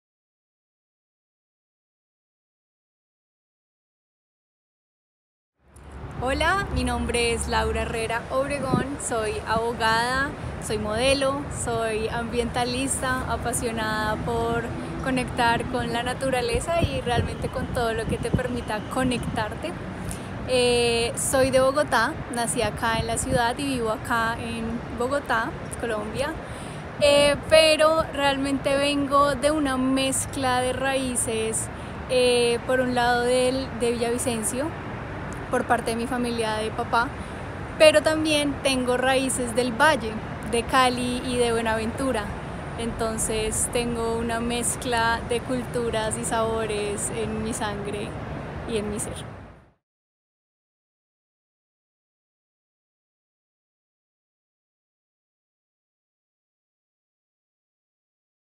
EPT: Presentación / Introducing Laura Herrera Obregon
Laura Herrera Obregon se presenta a la comunidad de EPT. Si quieres conocer la Anécdota de Pelos de Laura sigue este enlace

Laura Herrera Obregon introduces her self to the EPT community. If you want to watch her hair-anecdote follow this